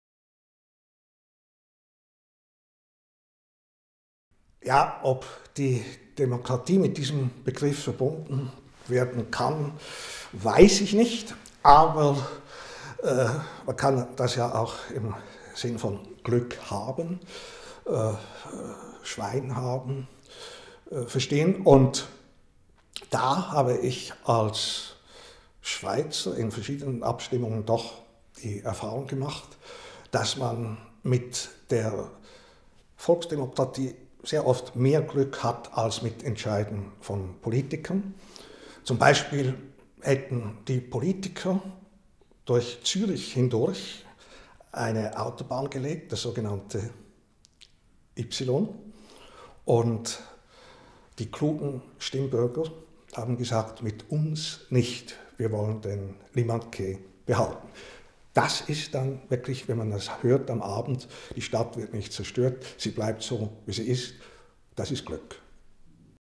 Thomas Hürlimann
Video-Statement von Thomas Hürlimann zum Thema Demokratie beim NZZ-Podium Stuttgart am 13. Juli 2017.